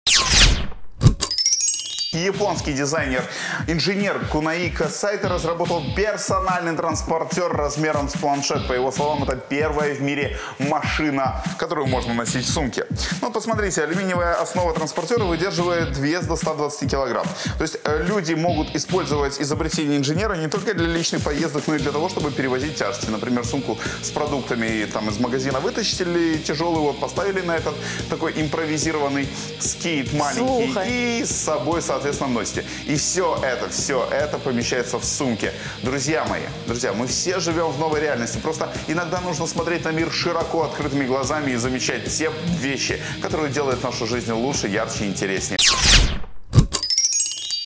В Японии изобрели первую «машину в сумке»
Японский инженер Кунаико Сайто разработал персональный транспортер размером с планшет. По словам создателя, это первая в мире «машина в сумке».